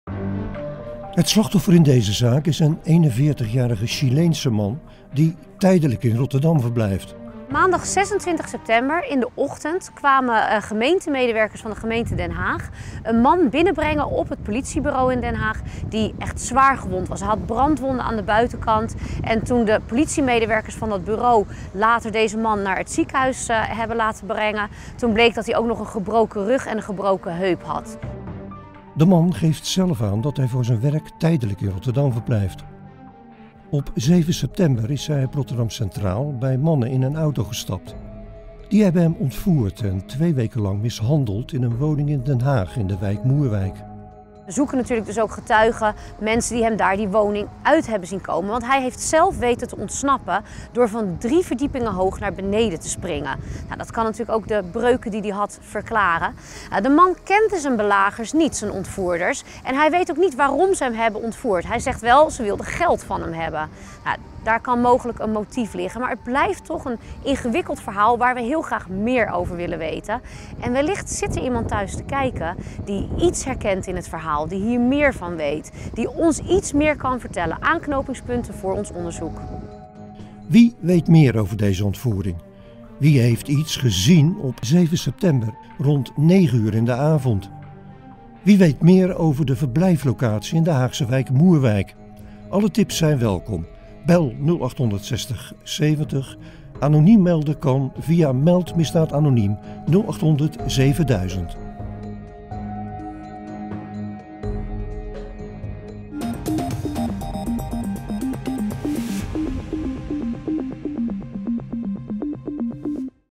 Chileense man springt uit raam en ontsnapt aan kidnappers
Ontvoerd en mishandeld wordt een Chileense man die niets vermoedend in een auto stapt die hem ophaalt op Rotterdam Centraal. Na ruim veertien dagen weet de man aan zijn kidnappers te ontsnappen door uit een raam van een woning in Den Haag te springen. De man wordt opgenomen in het ziekenhuis en daar blijkt dat hij zwaargewond is.

De 41-jarige man komt op woensdag 7 september omstreeks 20.30 uur met de trein aan in Rotterdam. Hij is hier voor zaken en stapt vervolgens in een auto, waar enkele mannen in zitten. De auto rijdt naar Den Haag waar de man in een woning in de Haagse wijk Moerwijk in de buurt van de Bellamystraat, Nieuwlandstraat, Pieter Langedijkstraat en de Betje Wolffstraat gevangen wordt gehouden en mishandeld. Volgens het slachtoffer eisen de ontvoerders geld.

De man klampt na zijn sprong op 26 september enkele medewerkers van Gemeentewerken aan die hem naar een politiebureau brengen. Het slachtoffer heeft verschillende brandwonden op zijn lichaam. In het ziekenhuis wordt geconstateerd dat de man ook een gebroken rug en een gebroken heup heeft opgelopen en hij wordt opgenomen. Hij heeft inmiddels het ziekenhuis verlaten, maar het herstel zal nog enige tijd duren.
Het slachtoffer heeft donker haar en een kort baardje. Op 26 september droeg hij een grijze broek, een grijze trui en zwarte sokken. De politie is op zoek naar meer informatie over de ontvoering. Mensen die camerabeelden hebben en/of iets hebben gezien in Rotterdam of Den Haag worden verzocht contact op te nemen.